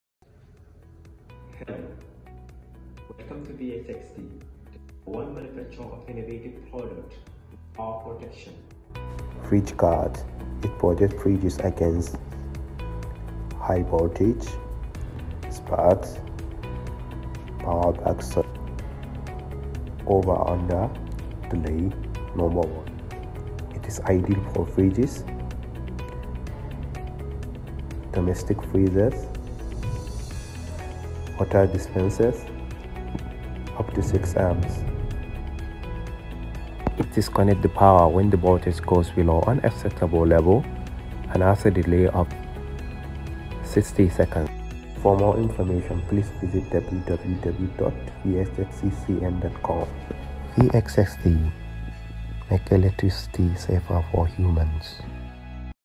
BXST-V187 voltage protector 220v household fridge guard tv guard
WENZHOU BXST CO.,LTD

BX-V187 is a household UK plug voltage protector. Applicable to TVs, computers, refrigerators and other types of electrical equipment with a power less than 2200W.
Under voltage protection: 150VAC
Over voltage protection: 260VAC
Maximum power/current: 2200W - 10A

Red light: overvoltage and undervoltage indicator
Yellow light: delay indicator
Green light: output indicator